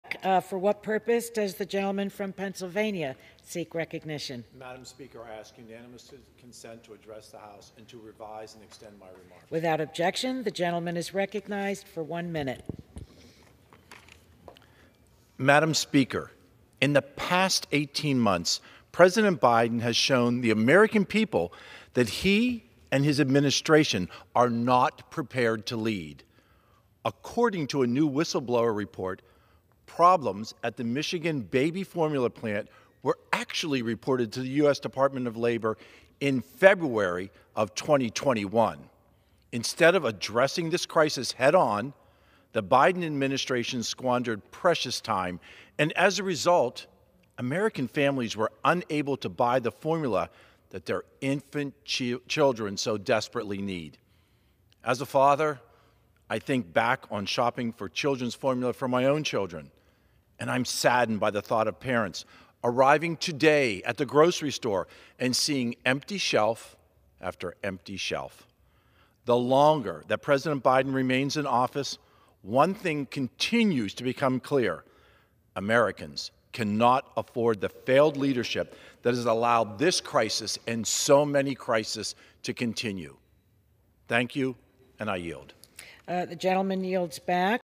‘He And His Admin Are Not Prepared To Lead’: John Joyce Slams Biden Over Infant Formula Shortage
During House Floor remarks last week, Rep. John Joyce (R-PA) decried the Biden Administration over the infant formula shortage.